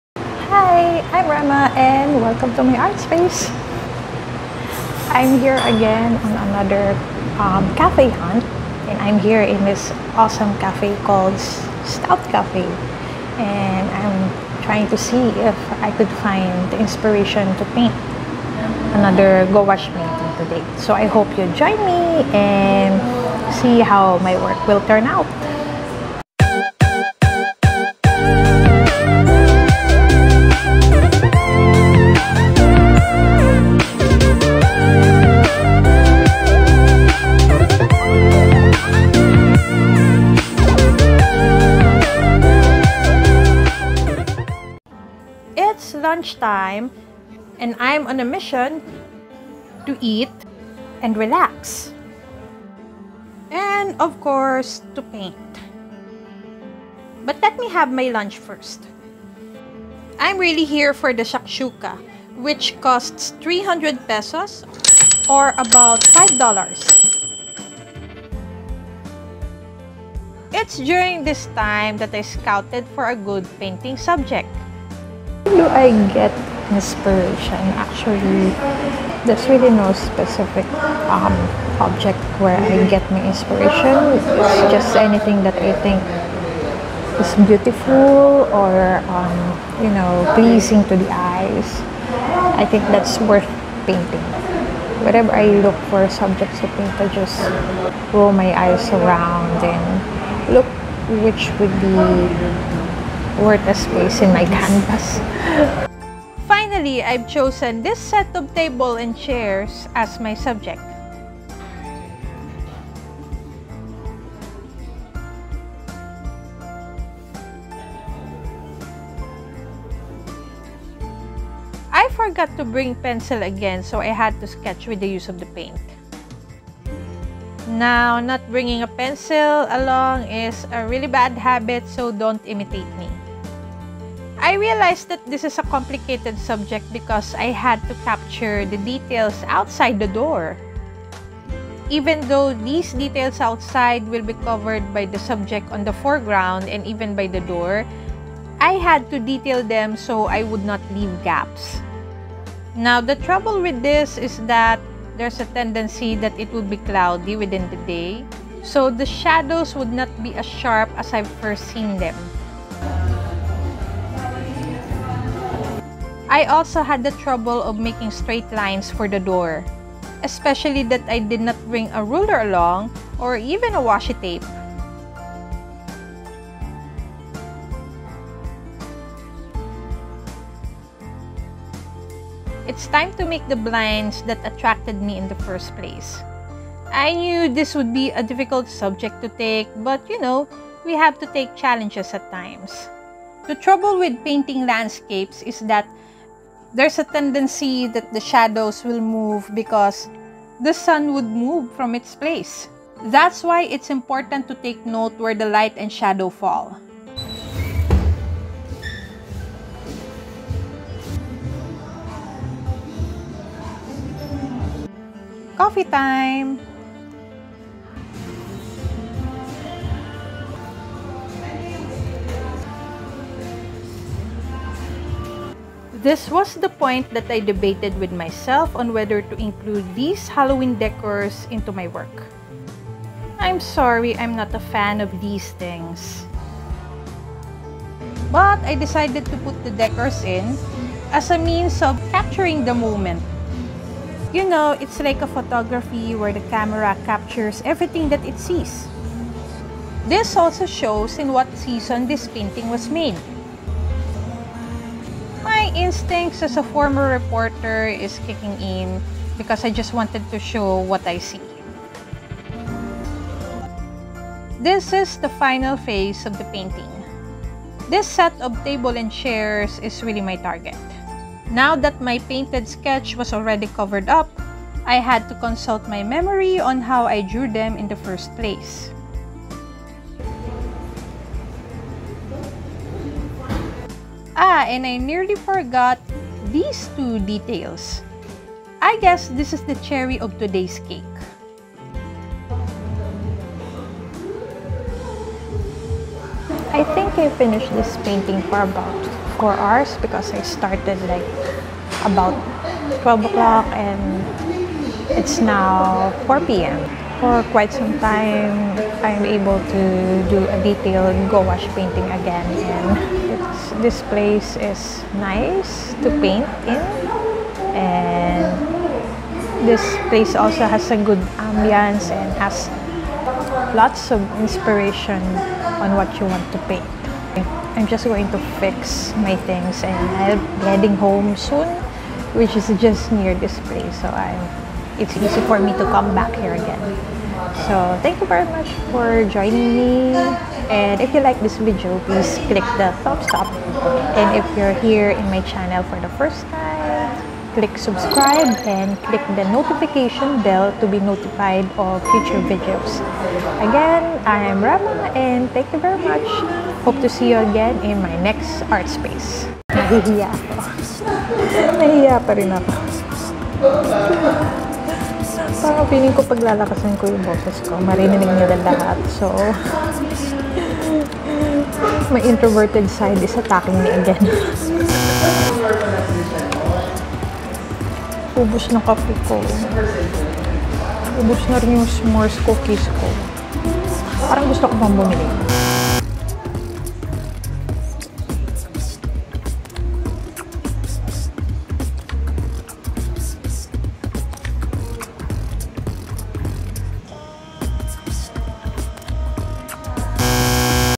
Gouache painting at Stout Cafè
I'm back at hunting cafès again to look for painting inspiration. Stout Cafè at Sta. Maria, Bulacan, Philippines is a good starting point to make another gouache painting, especially that they have good food and ambience.